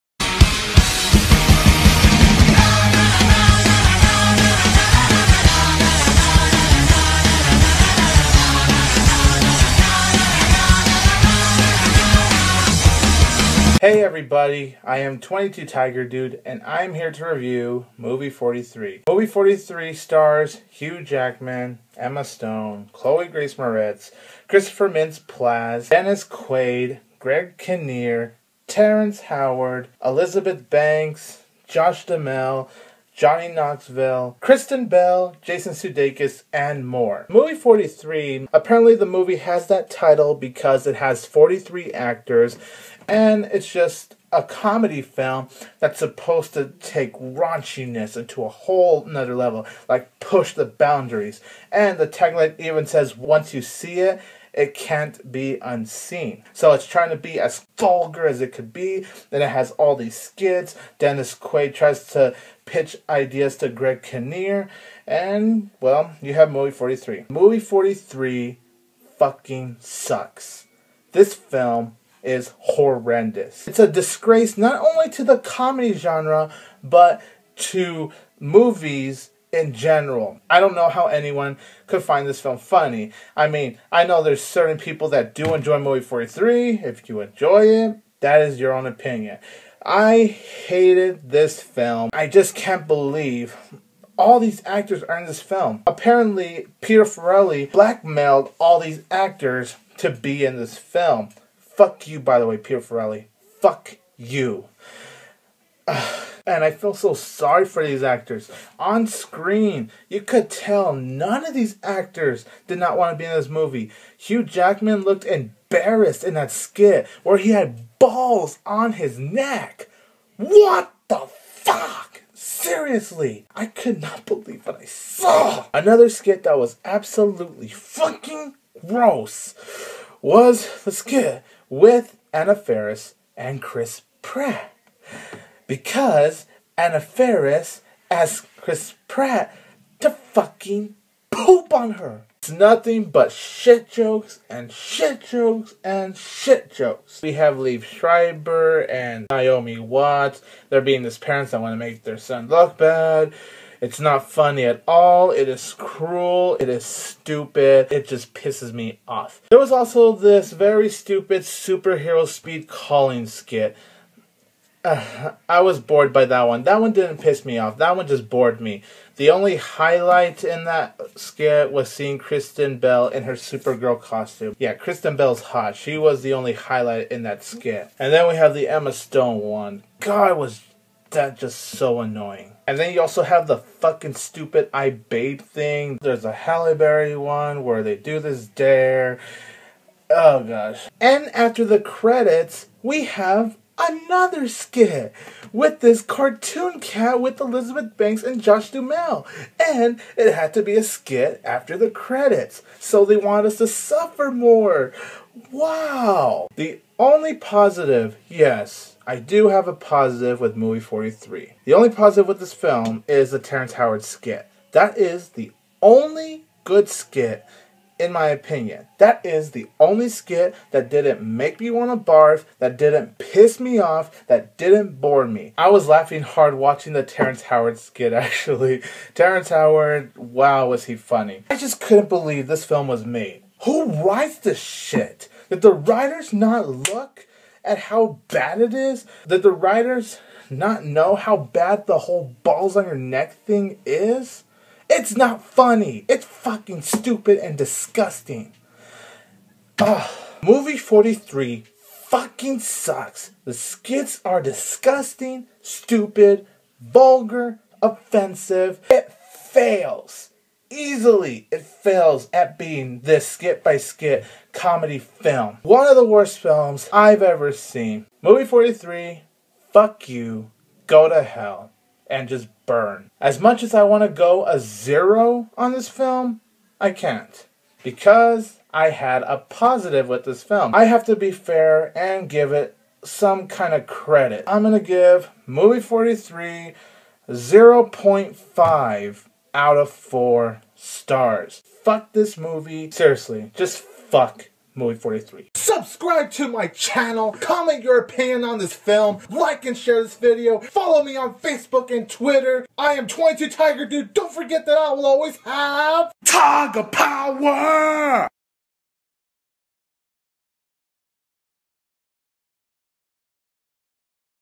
Movie 43 movie review
22tigerdude reviews the skit by skit R-rated raunchy film with the biggest assembled cast ever. What are his thoughts on this film? find out here.